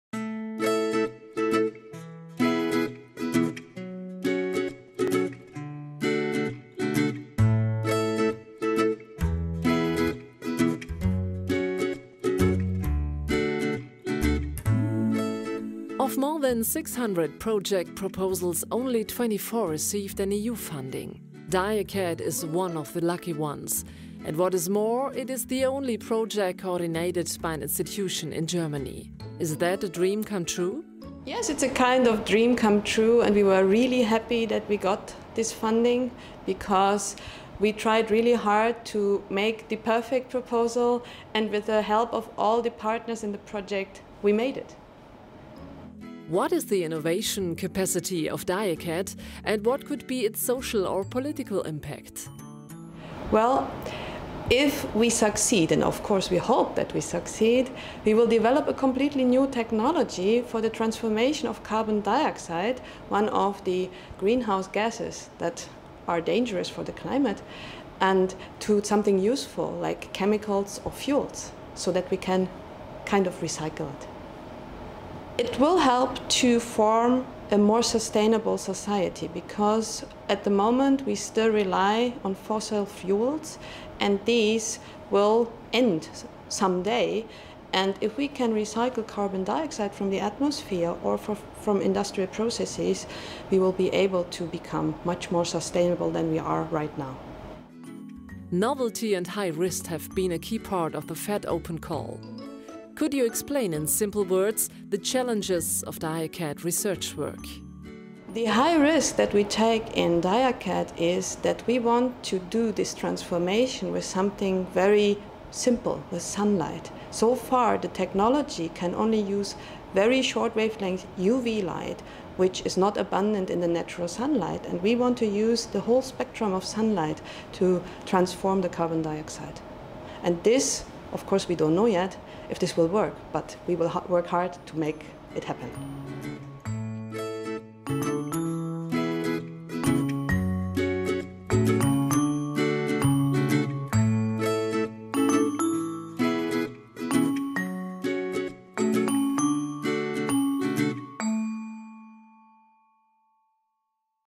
DIACAT Project - Prof. Anke Krueger (Project Coordinator), University of Würzburg
The multinational collaborative FET-Open project DIACAT aims to develop a completely new technology for the direct photocatalytic conversion of CO2 into fine chemicals and fuels using visible light. The innovative approach utilises the unique property of man-made diamond, now widely available at low economic cost, to generate solvated electrons upon light irradiation in solutions (e.g. in water and ionic liquids).